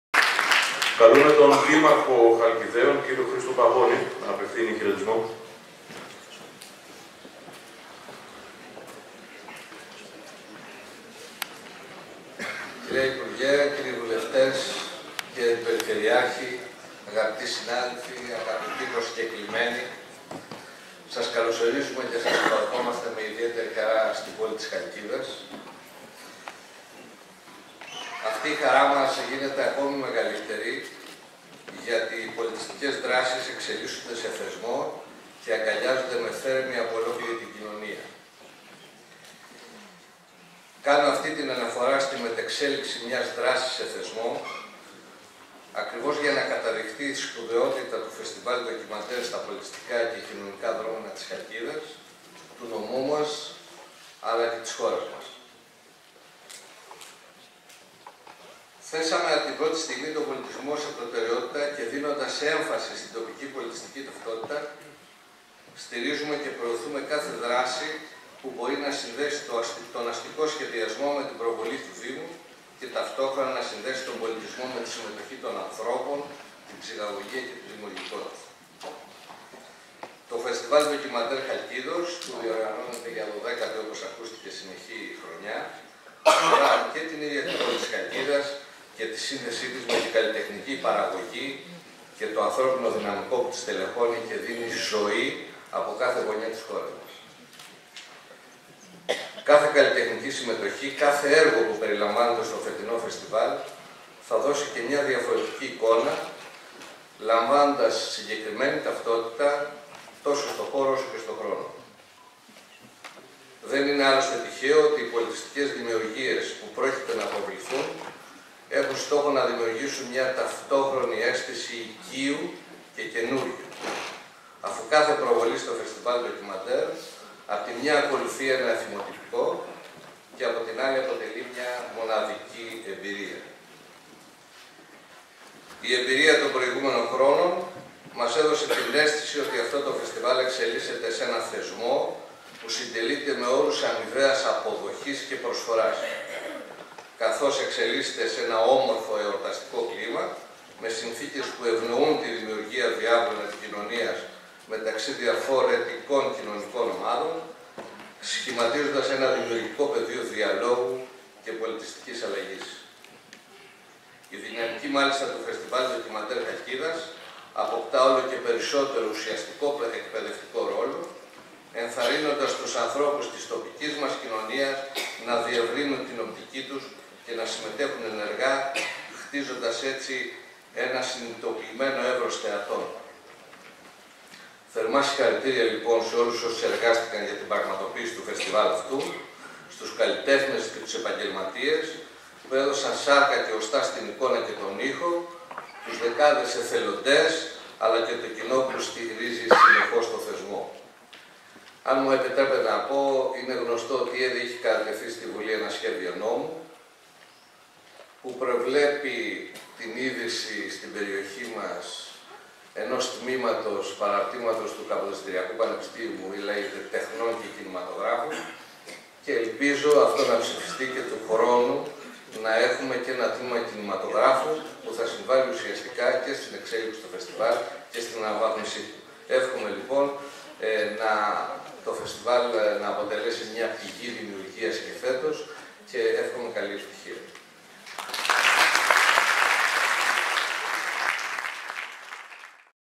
12ο Φεστιβάλ Ελληνικού Ντοκιμαντέρ στη Χαλκίδα 13-18/11/2018
Χαιρετισμός από τον Δήμαρχο Χαλκιδέων Χρήστο Παγώνη
Για πληροφορίες πατήστε εδώ :